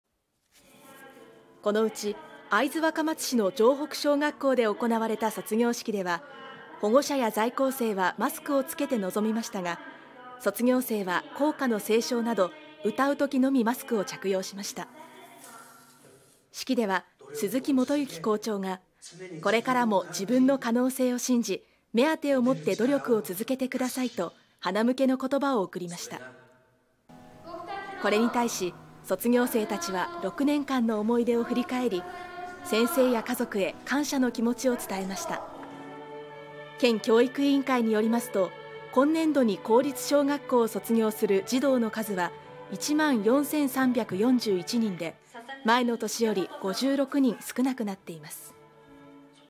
公立小学校で卒業式 1万4000人学び舎あとに 保護者や在校生はマスク着用 福島
23日、福島県内のほとんどの公立小学校で、卒業式が行われ、1万4000人あまりの児童が通いなれた学び舎を後にします。このうち、会津若松市の城北小学校で行われた卒業式では、保護者や在校生はマスクを着けて臨みましたが、卒業生は、校歌の斉唱など歌う時のみマスクを着用しました。式では、鈴木基之校長が「これからも自分の可能性を信じ、目当てを持って努力を続けてください」とはなむけの言葉を贈りました。これに対し、卒業生たちは6年間の思い出を振り返り、先生や家族へ感謝の気持ちを伝えました。卒業生「文武両道として部活とか体育は苦手なんですけど、体育も頑張って、勉強も頑張っていきたいと思う」
卒業生「卒業したなという感じがあって、それと一緒に悲しみもありました」
卒業生「仲間と過ごした6年間が本当に楽しくて、これからいなくなっちゃう子もいるので寂しいなという思いと、これから人生の一歩を踏み出せたのかなと思う」県教育委員会によりますと、今年度に公立小学校を卒業する児童の数は1万4341人で、前の年より56人少なくなっています。